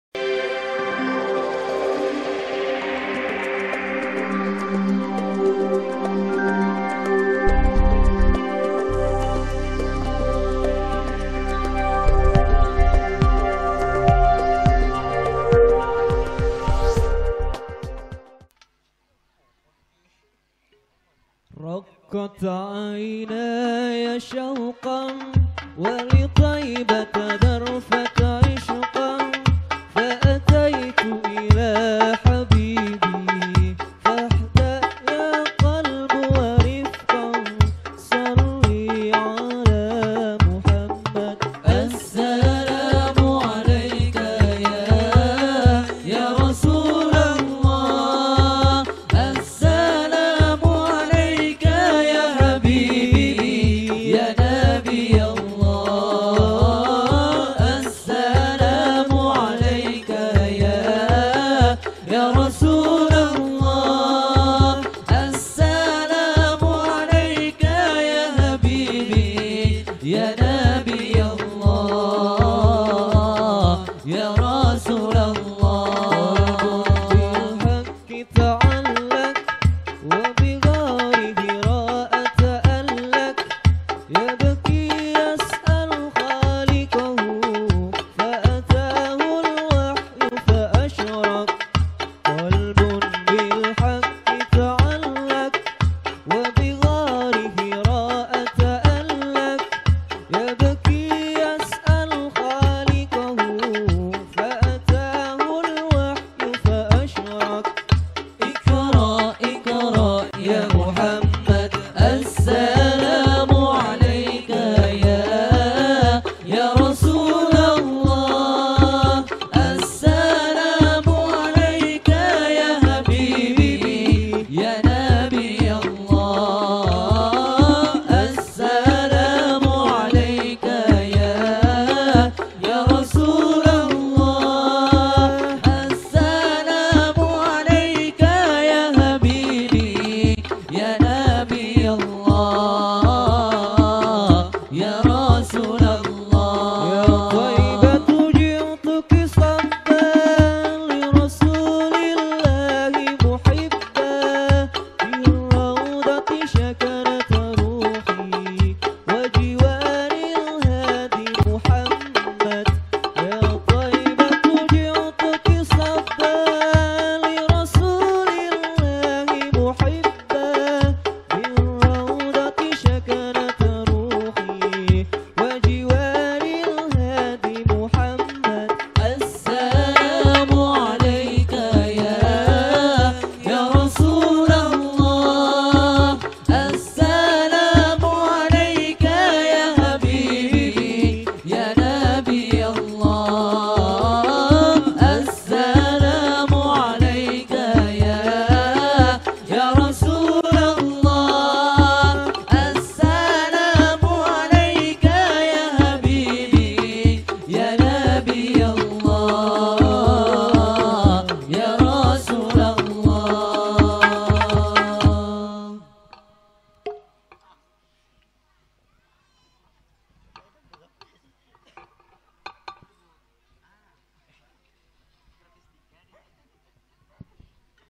INDAHNYA BUNYI MUSIK TRADISIONAL SAAT DITABUH SANG PROFESIONAL
SHOLAWAT TRADISIONAL MUSIK JEMBER STMJ RUSYDA. DALAM ACARA SENANDUNG CINTA UNTUK SANG NABI DAN RA BAITUL AMIEN BERBAGI SANTUNAN ANAK YATIM DAN KAUM DUAFA. HAFLAH AKHIR SANAH XXI YAYASAN BAITUL AMIN AL BAROKAH. GENDIR KARANGPRING SUKORAMBI JEMBER.